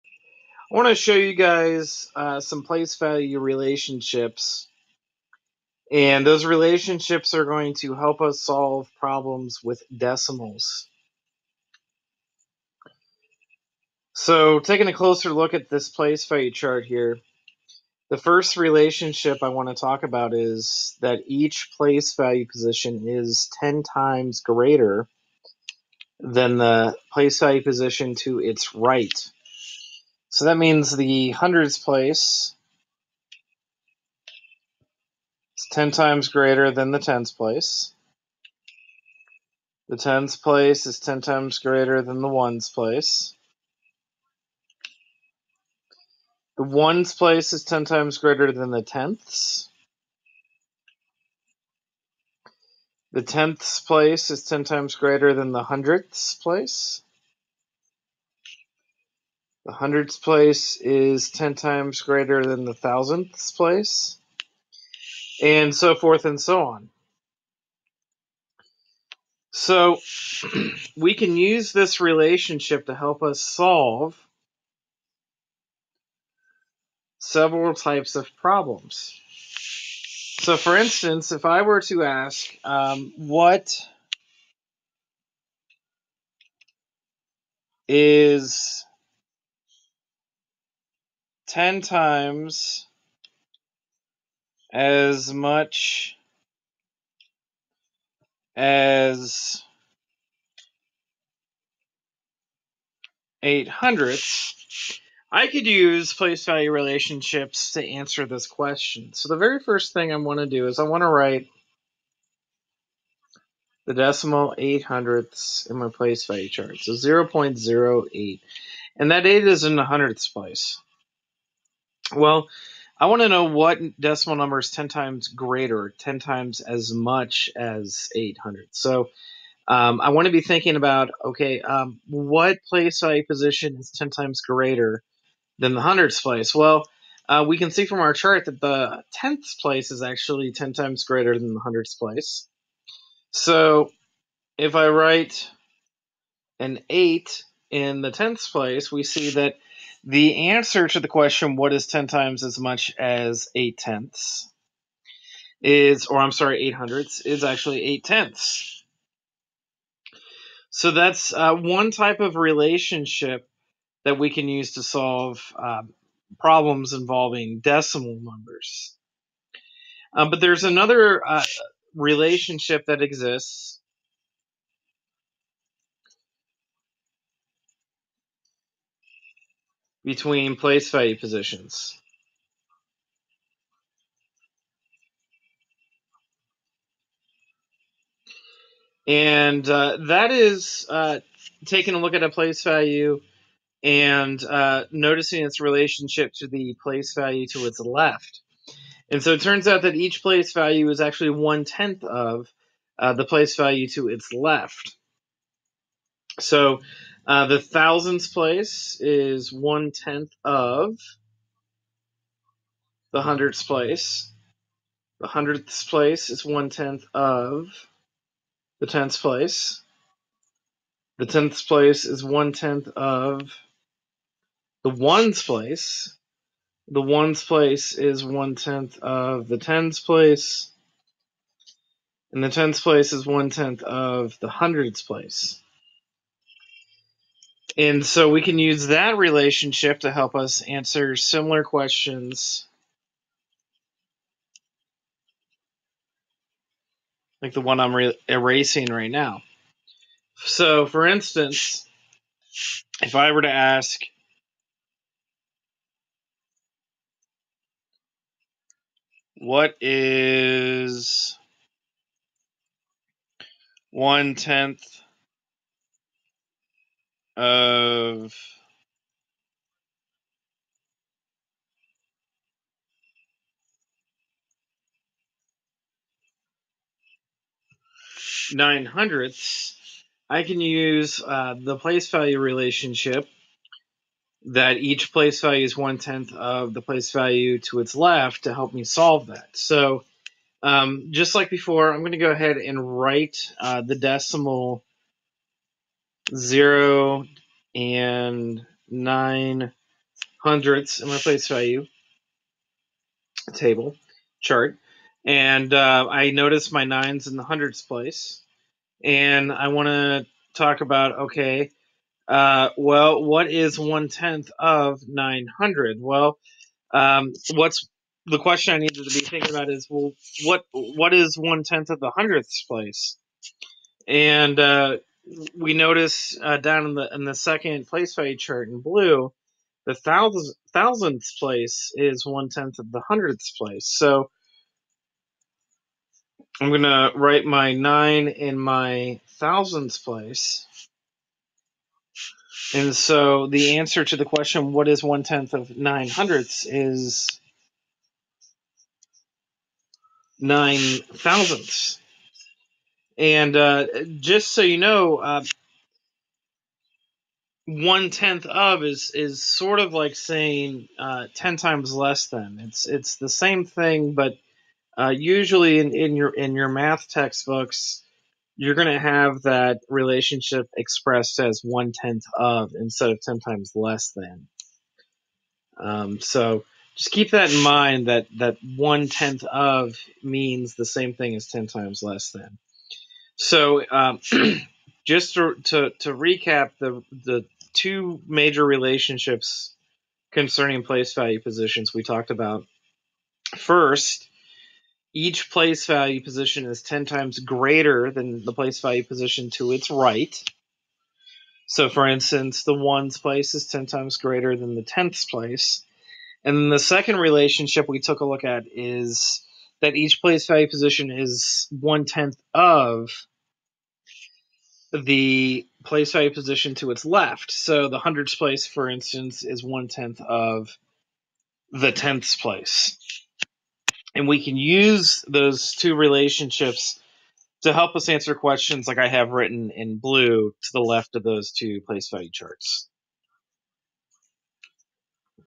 Place Value Relationships with Decimals
This video shows how place value relationship can be used to solve questions involving decimals.